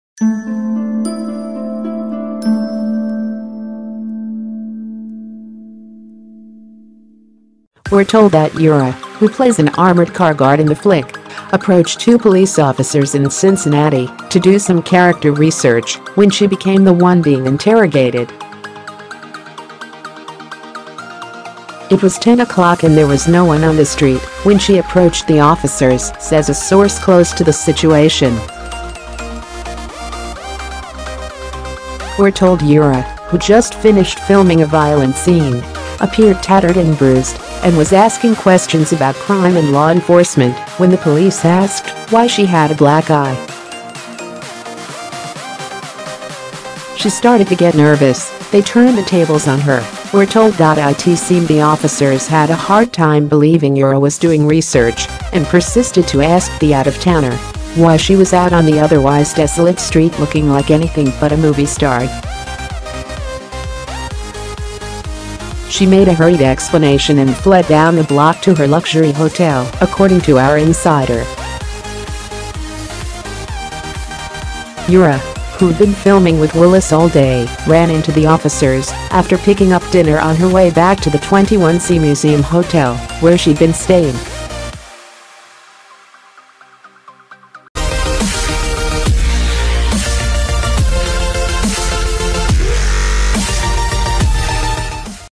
'Bruised' Bruce Willis co-star gets cops' attention
We’re told that Yura, who plays an armored car guard in the flick, approached two police officers in Cincinnati to do some character research when she became the one being interrogated. “It was 10 o’clock and there was no one on the street when she approached the officers,” says a source close to the situation. We’re told Yura, who’d just finished filming a violent scene, appeared tattered and bruised and was asking questions about crime and law enforcement when the police asked why she had a black eye. “She started to get nervous — they turned the tables on her,” we’re told.It seemed the officers had a hard time believing Yura was doing research and persisted to ask the out-of-towner why she was out on the otherwise desolate street looking like anything but a movie star. “She made a hurried explanation and fled down the block to her luxury hotel,” according to our insider. Yura, who’d been filming with Willis all day, ran into the officers after picking up dinner on her way back to the 21c Museum Hotel, where she’d been staying.